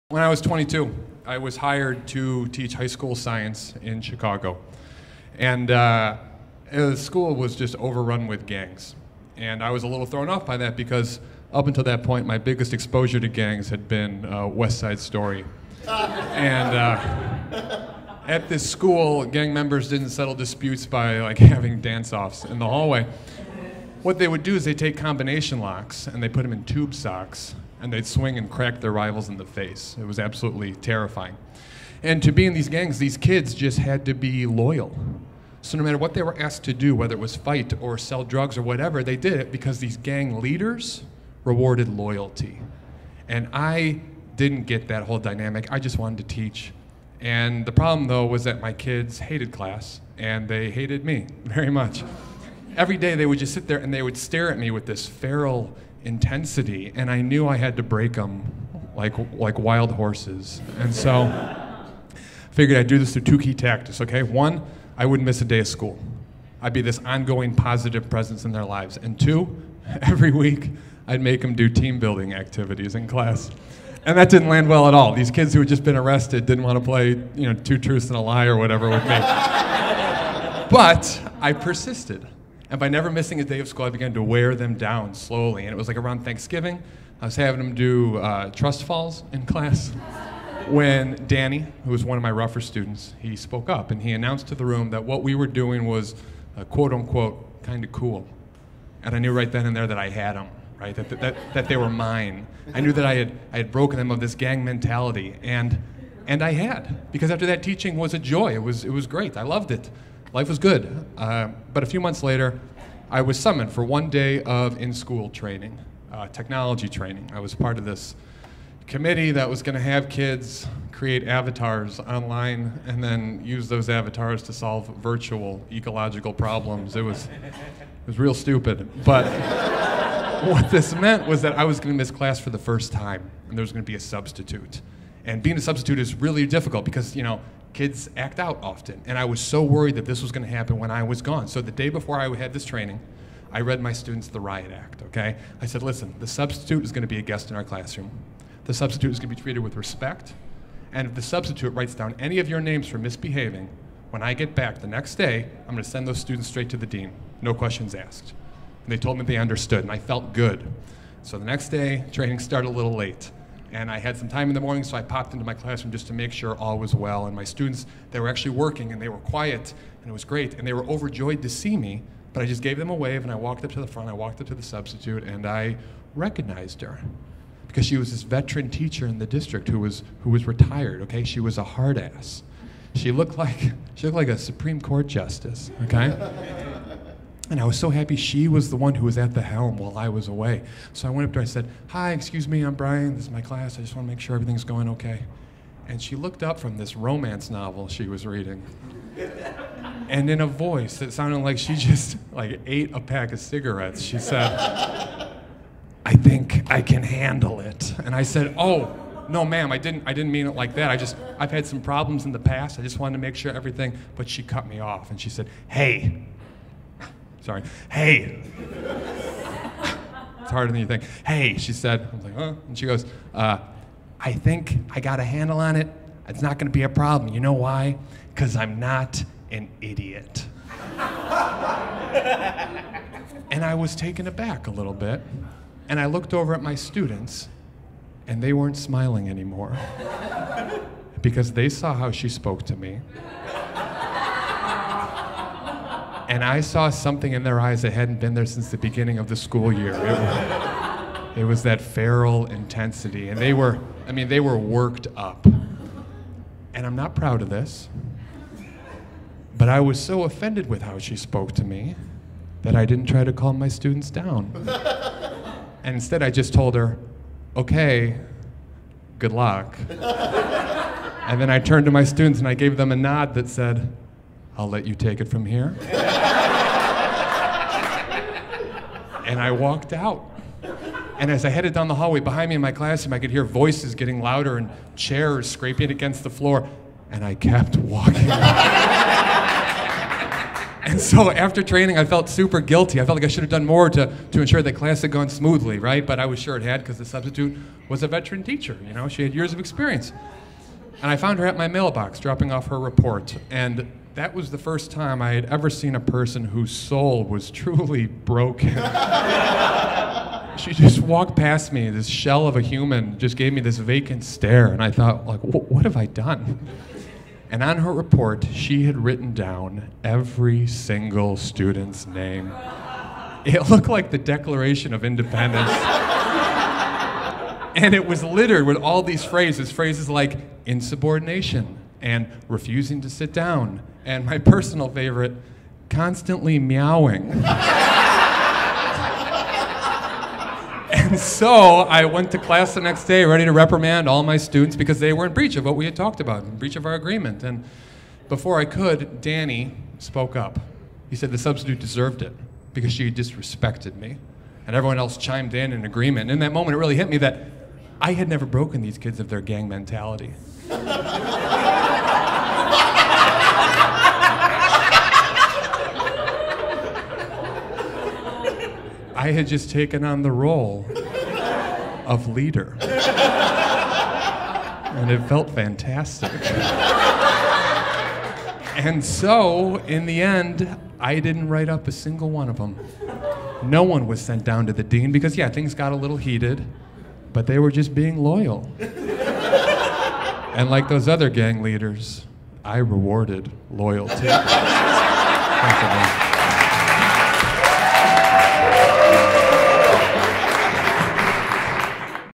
Bryan Kett Becomes a Gang Leader
Bryan Kett leads a Chicago high school away from gangs...and ends up becoming their leader.

This story may not be true. Think it's a lie?